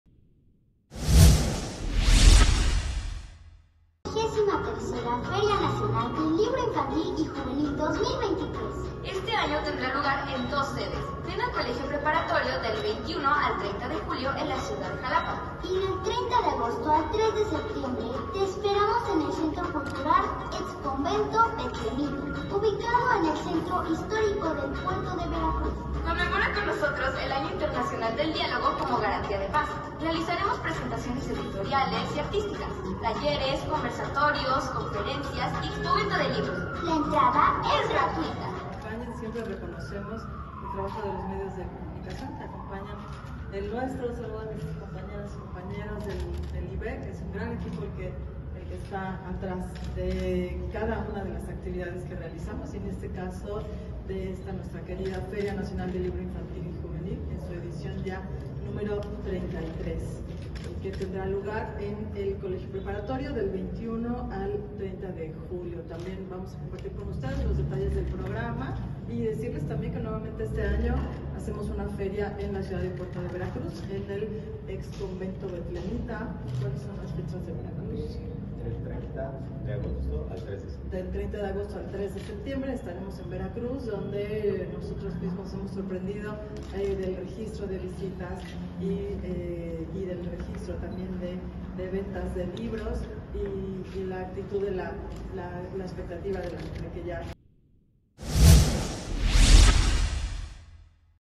Regresa a la “Prepa Juárez” la Feria del Libro Infantil y Juvenil
La directora de Instituto Veracruzano de la Cultura (IVEC), Silvia Alejandre Prado, encabezó la presentación de la trigésima tercera edición de la Feria Nacional del Libro Infantil y Juvenil en Xalapa, a inaugurarse el 21 de julio.